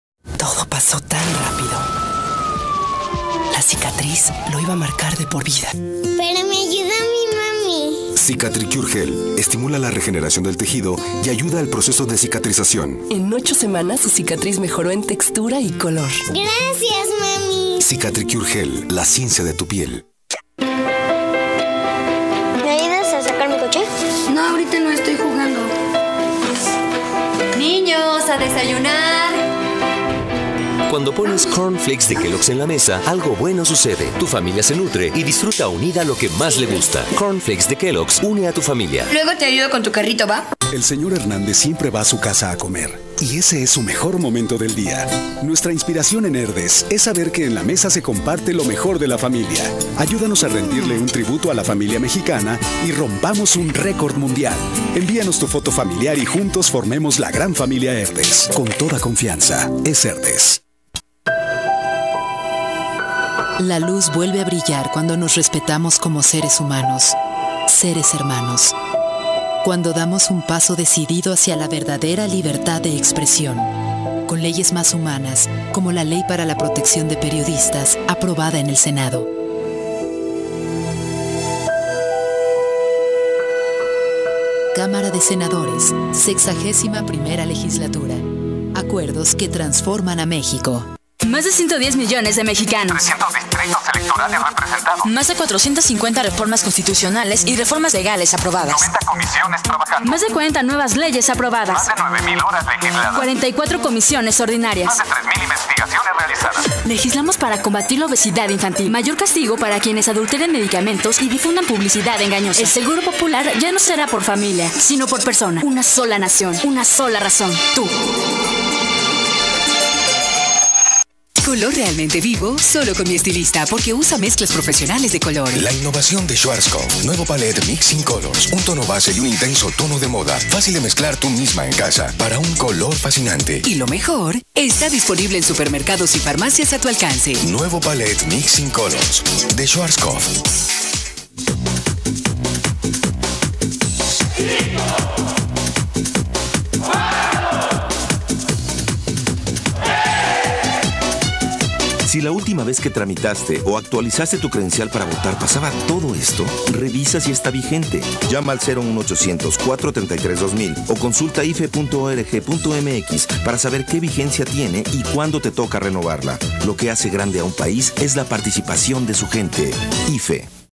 Comerciales Julio 2012 Feed Tampico
son los anuncios que tengo en julio 2012 tampico los voy a subir Comerciales radio agosto 2009 feliz día de las madres